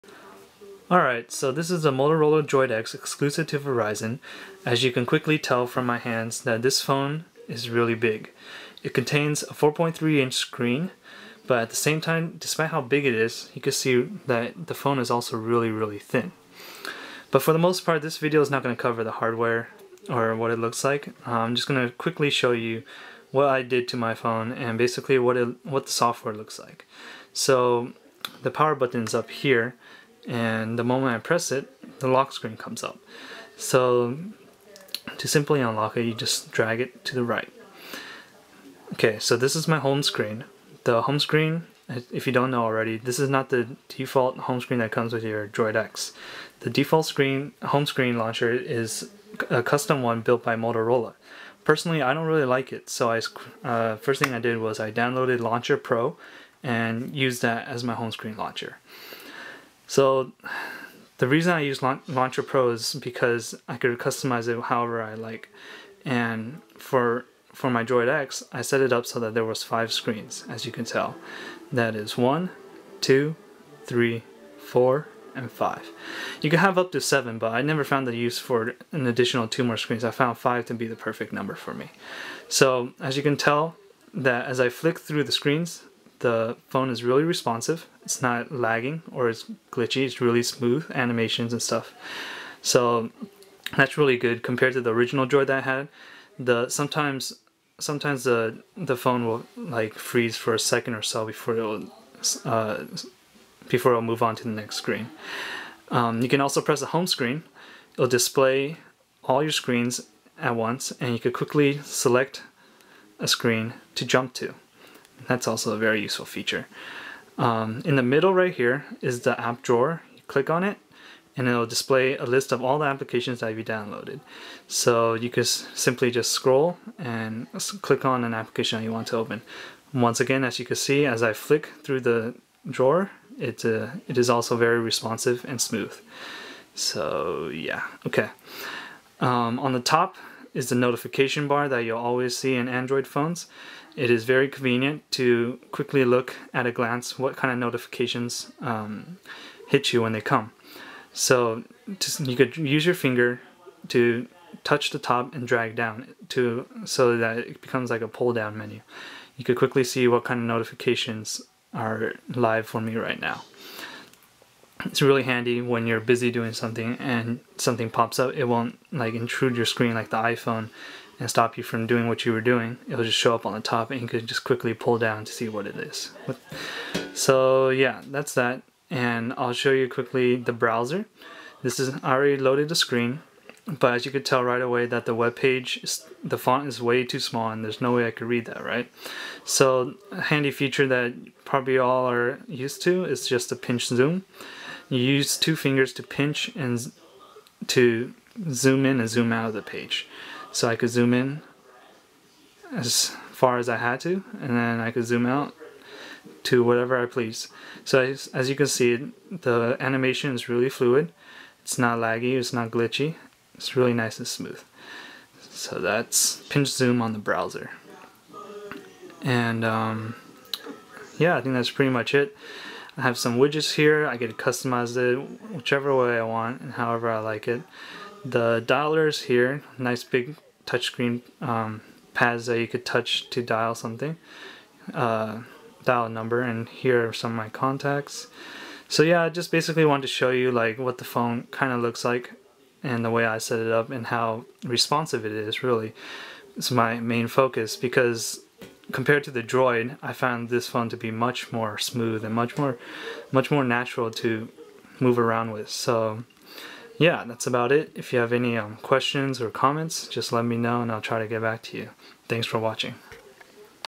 Motorola Droid X Software Overview - LauncherPro
A VERY brief overview of the software on the Motorola Droid X.

The default home launcher on the Droid X is a custom one made by Motorola that included their widgets.  I replaced it with LauncherPro and gave a short look at it in this video along with the basic functionalities of the android OS.